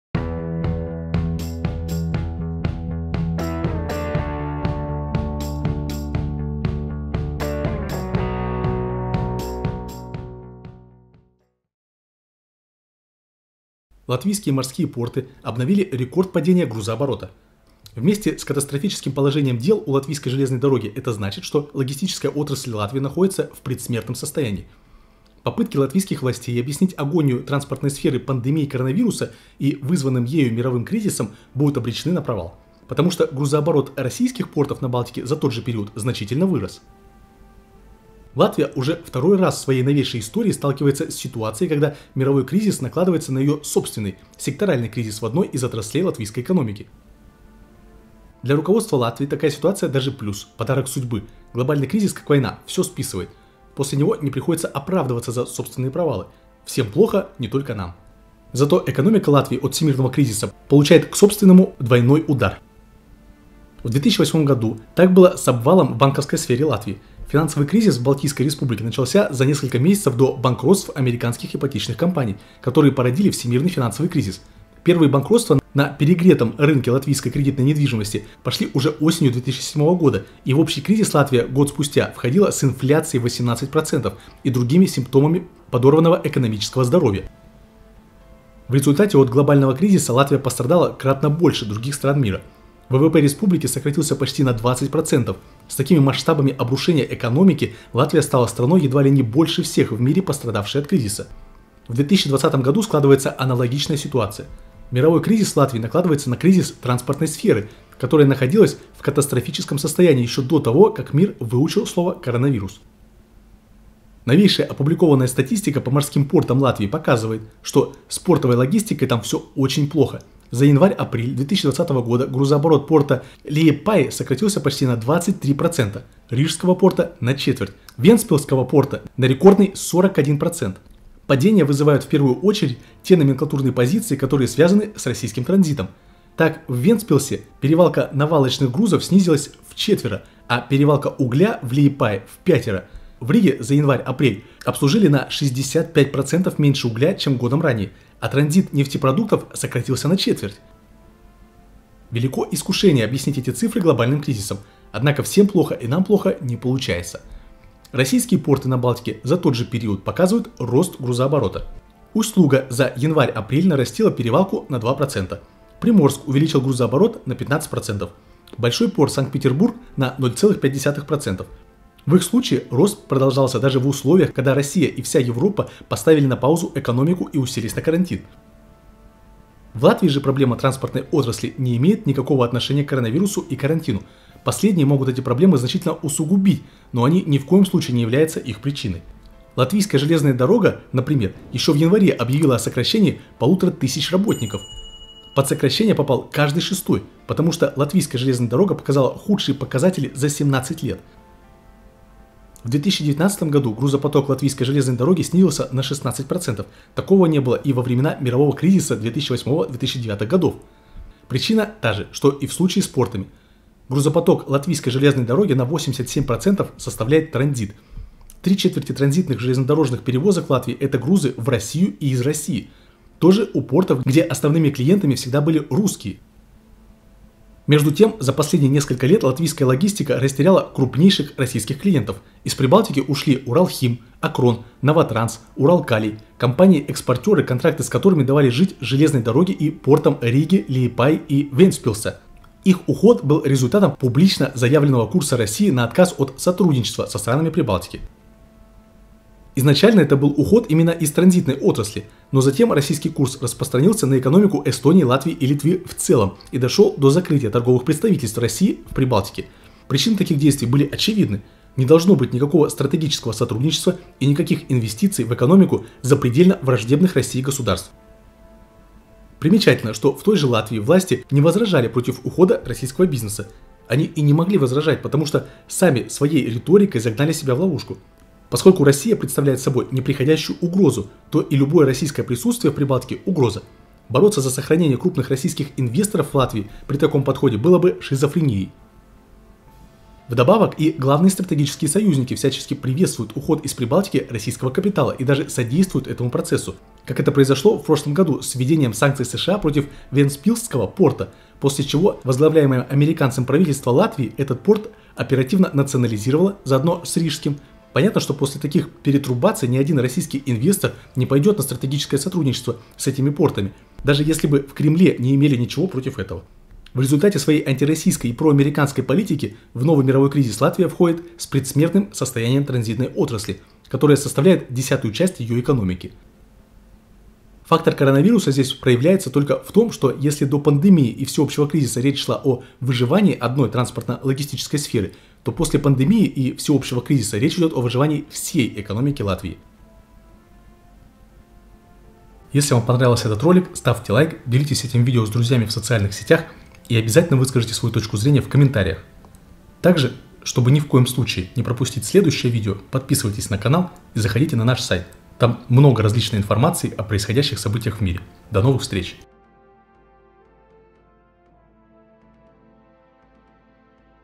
Рекорд падения! Россия и коронавирус добивают порты Латвии
Благодарим за добровольную помощь в развитии канала:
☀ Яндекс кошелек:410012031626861